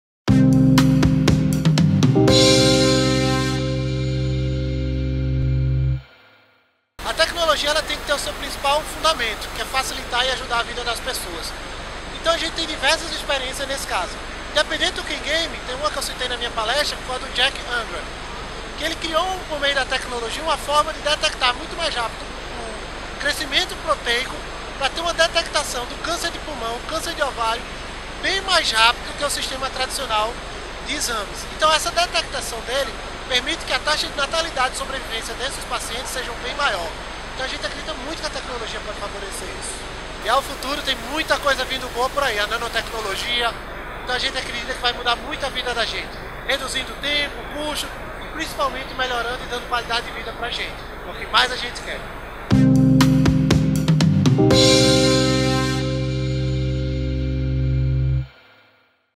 O uso da tecnologia na saúde é debatido na Campus Party Brasília
O principal fundamento da tecnologia é ajudar e facilitar a vida das pessoas. Com isso em mente, o engenheiro de software Eraldo Guerra criou o CanGame – plataforma multidisciplinar para o tratamento de crianças autistas. O engenheiro acredita que a saúde será beneficiada pelas novas tecnologias cada vez. Eraldo foi um dos palestrantes da Campus Party Brasília.